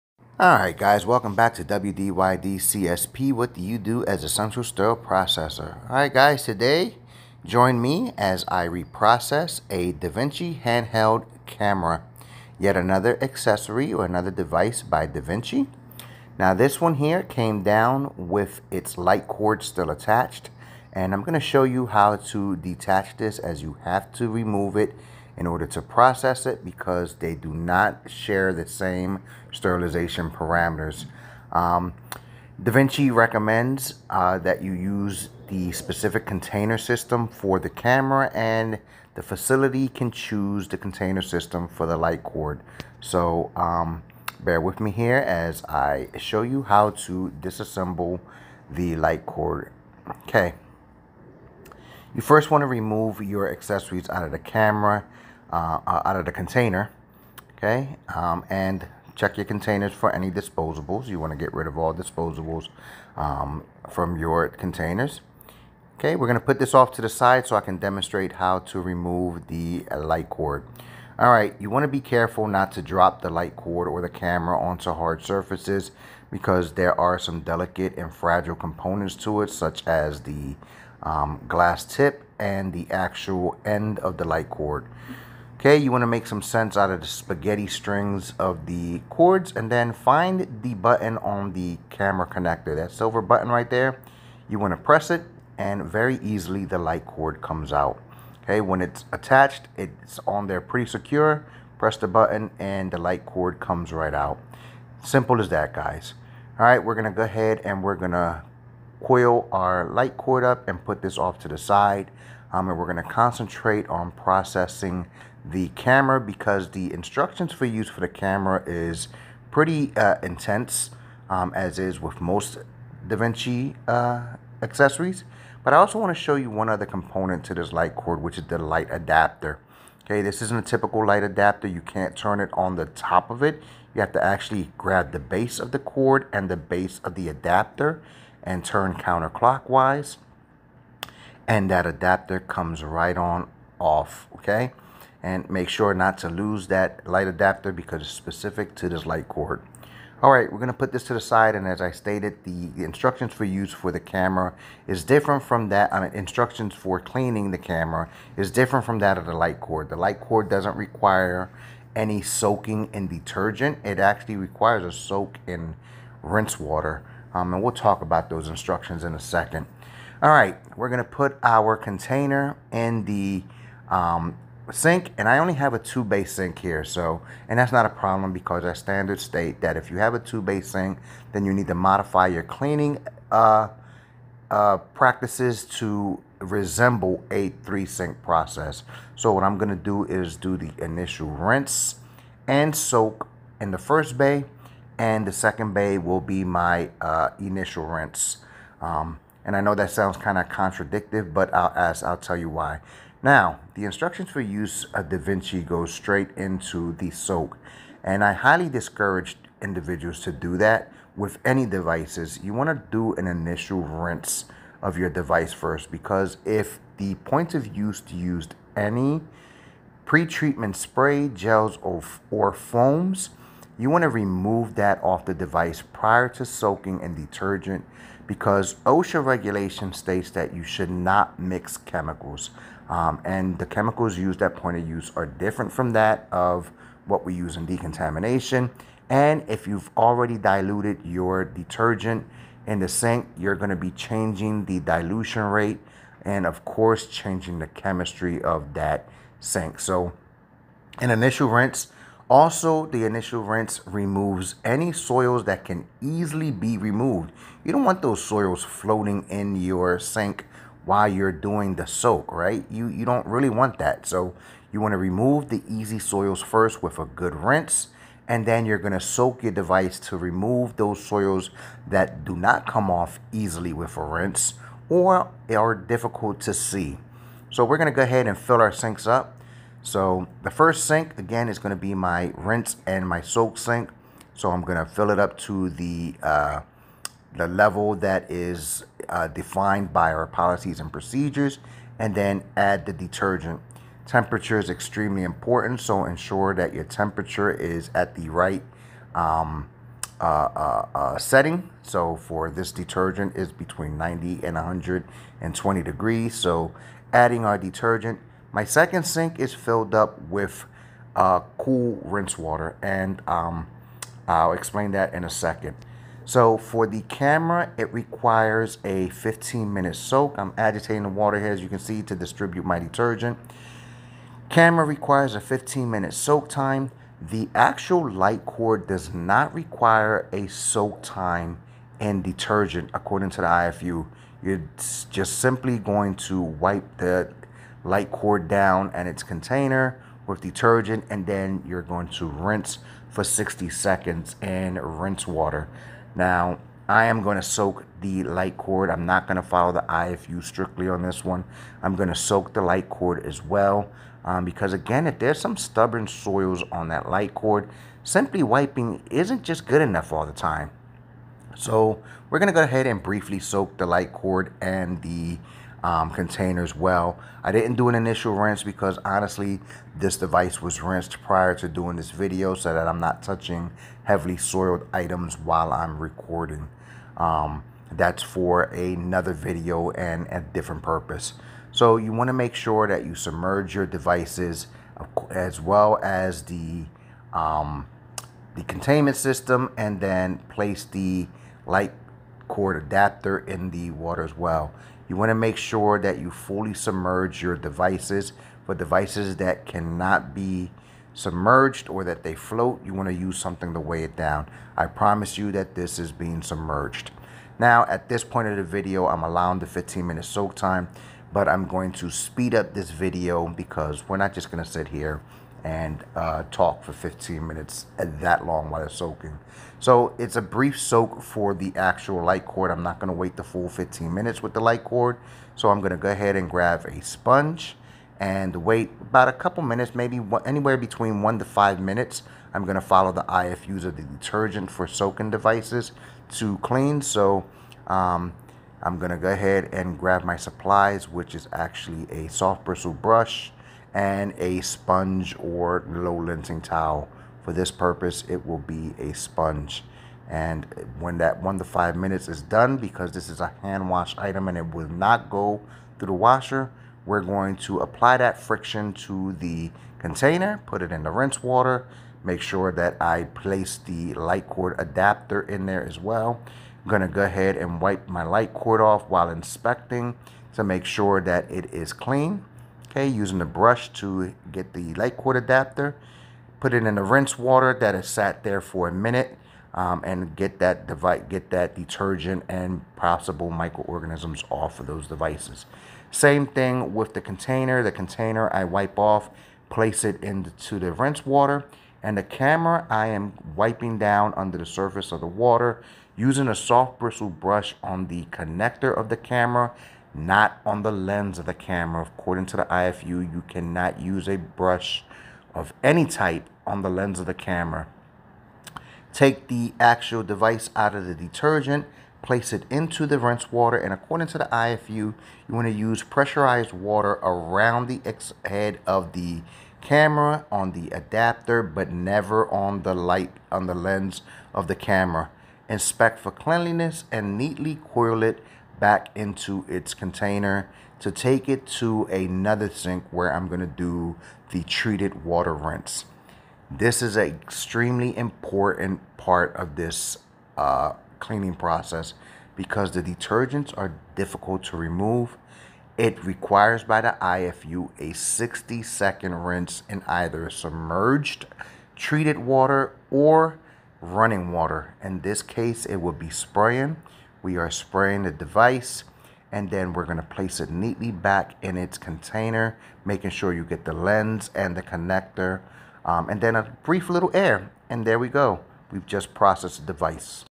Sterile Proceesing: Cleaning the DaVinci handheld camera and light cord
free SPD education